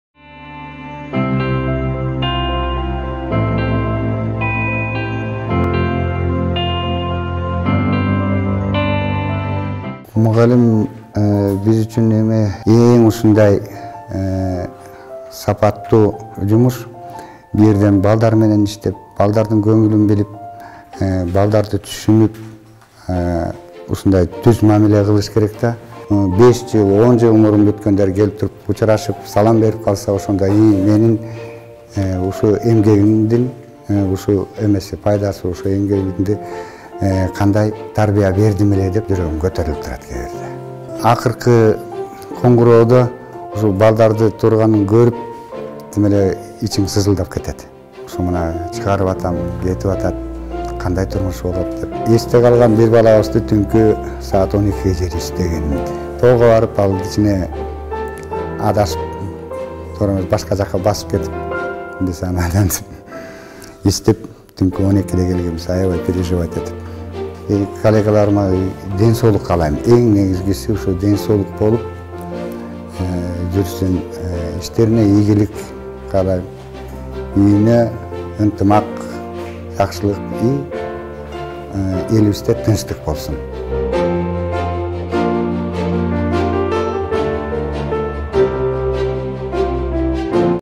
Марлес Сарбашев: Окуучуларым жакшы чыкса телегейим тегиз болот
В Кыргызстана отмечают ежегодно отмечают День учителя.
По словам президента Кыргызстана Алмазбек Атамбаева, в нынешних условиях одной из важнейших задач государства является воспитание нового гражданина Кыргызстана. Интеллектуально и физически развитого. Грамотного, здорового, свободно владеющего несколькими языками. Счастливого и успешного.
«На учителя возложена важнейшая миссия стать первым после родителей проводником будущего гражданина по лабиринтам жизни. Именно вы открываете перед нашими детьми двери к свету знаний, формируете первые представления о Добре и Зле», - сказал президент Кыргызстана Алмазбек Атамбаев.